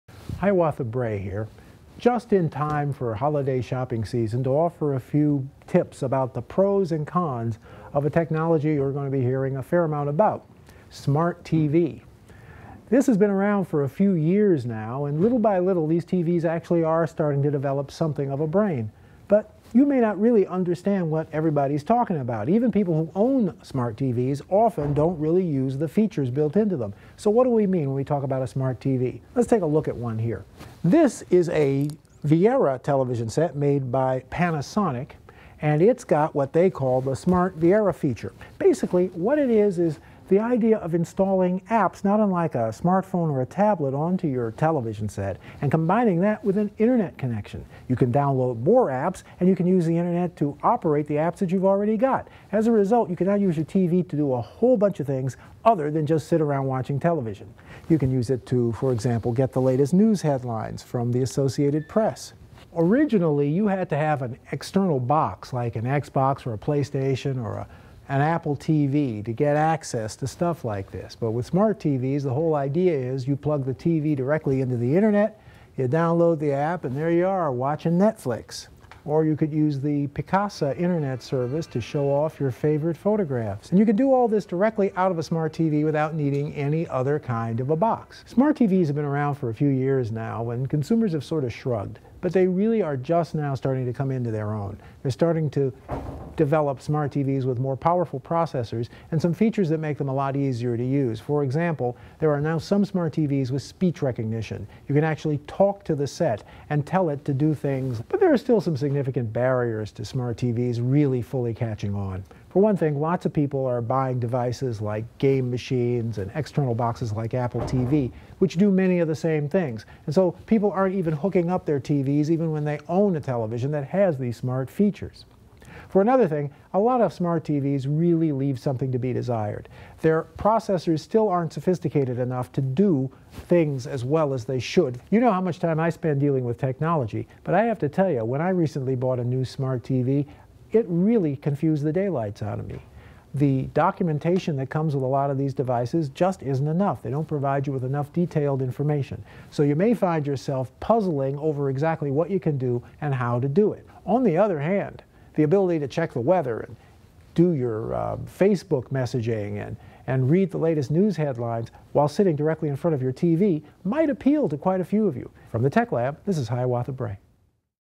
Tech Lab: smart TV
(Boston Globe) In this episode of Tech Lab, Hiawatha Bray talks about the features of "smart TV."